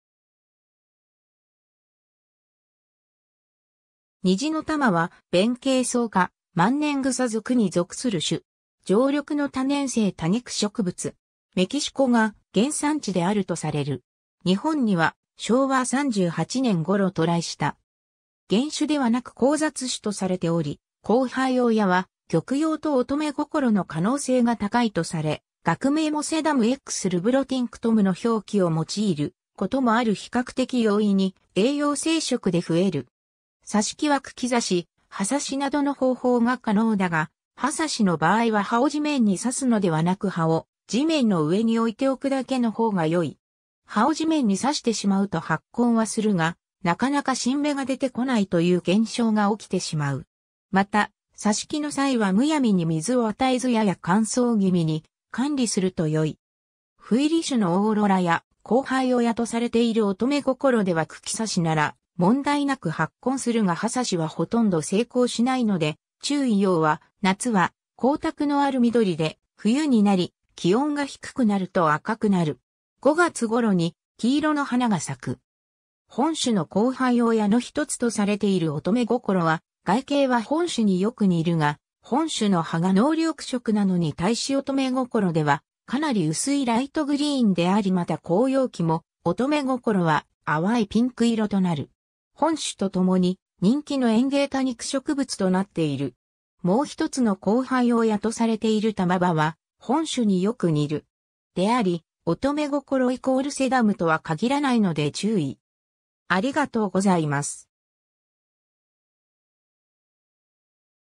ニジノタマ
ニジノタマ(Sedum rubrotinctum)は、ベンケイソウ科、マンネングサ属に属する種。
常緑の多年生多肉植物。
 メキシコが原産地であるとされる。
日本には、昭和38年頃渡来した。
 原種ではなく交雑種とされており、交配親は玉葉(S. stahlii)と乙女心(S. pachyphyllum)の可能性が高いとされ学名もSedum X rubrotinctumの表記を用いることもある 比較的容易に栄養生殖で増える。
挿し木は茎挿し、葉挿しなどの方法が可能だが、葉挿しの場合は葉を地面に挿すのではなく葉を地面の上に置いておくだけの方が良い。
葉を地面に挿してしまうと発根はするがなかなか新芽が出てこないとゆう現象が起きてしまう。
また、挿し木の際は無暗に水を与えずやや乾燥気味に管理すると良い。
斑入り種のオーロラや交配親とされているオトメゴコロでは茎挿しなら問題なく発根するが葉挿しは殆ど成功しないので注意（葉緑素の保有率が低いのが関係しているのではないかとされている） 葉は、夏は光沢のある緑で、冬になり気温が低くなると赤くなる。
5月ごろに黄色の花が咲く。
 本種の交配親のひとつとされている乙女心(S. pachyphyllum)は外形は本種によく似るが、本種の葉が濃緑色なのに対し乙女心では、かなり薄いライトグリーンでありまた紅葉期も乙女心は淡いピンク色となる。
本種とともに人気の園芸多肉植物となっている。
もうひとつの交配親とされているタマバは本種によく似る。
 （※乙女心とゆう品種名は多くの植物に使われている品種名（特にバラが有名）であり、乙女心＝セダムとは限らないので注意。